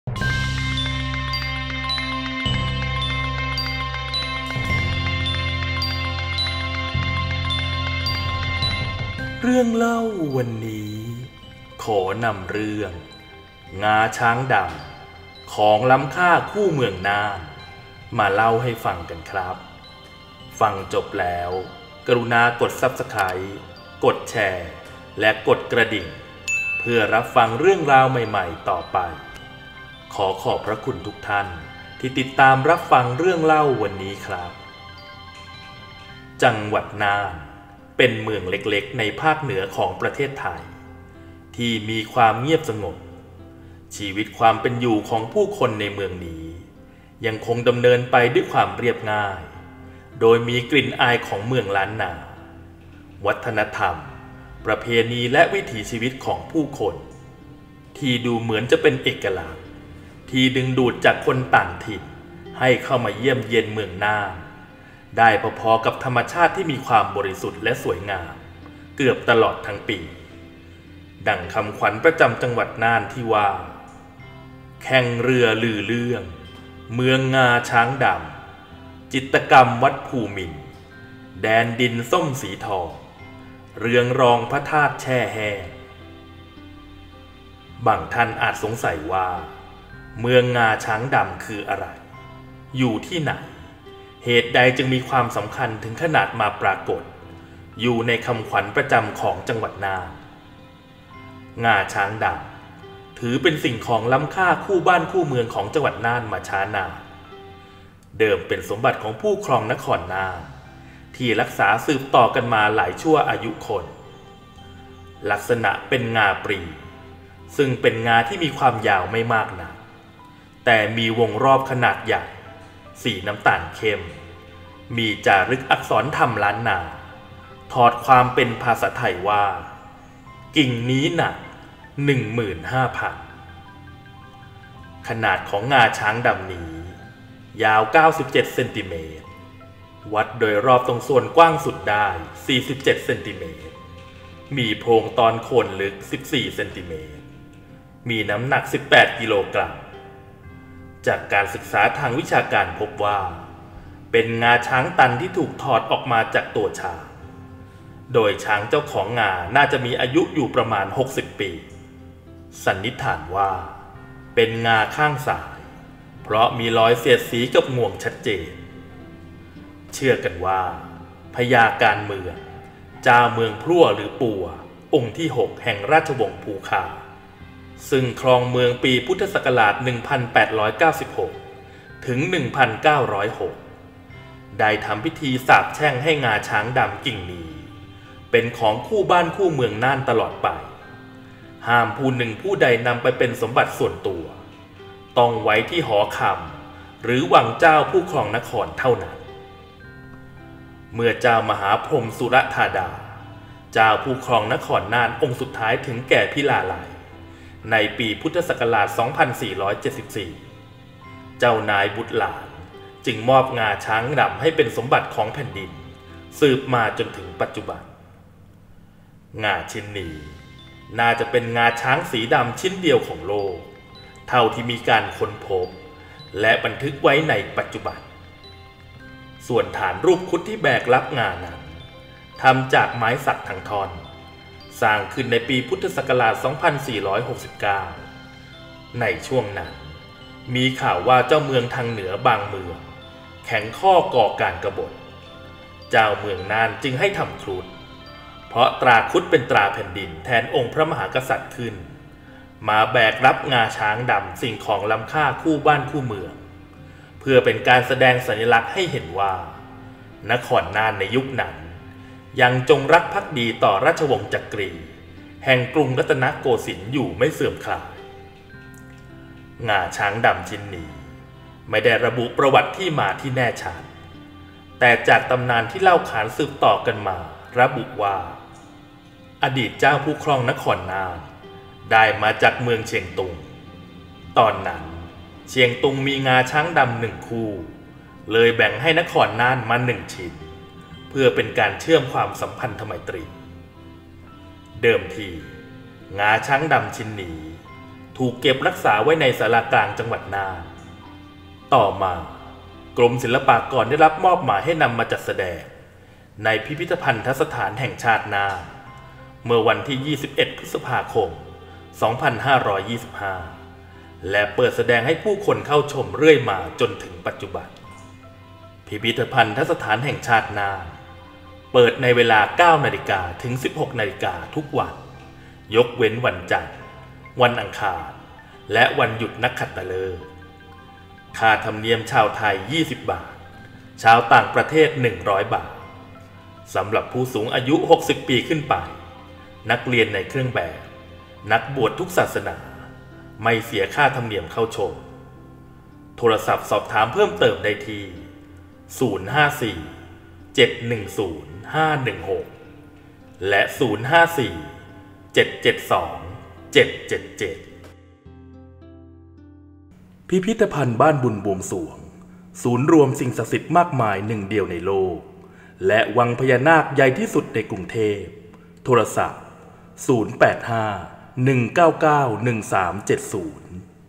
งาช้างดำ ของล้ำค่าคู่เมืองน่าน | เรื่องเล่าวันนี้
เรื่องเล่าวันนี้ขอนำเสนอเรื่องราวของ งาช้างดำ ของล้ำค่าคู่เมืองน่าน มาเล่าให้ฟังกันครับ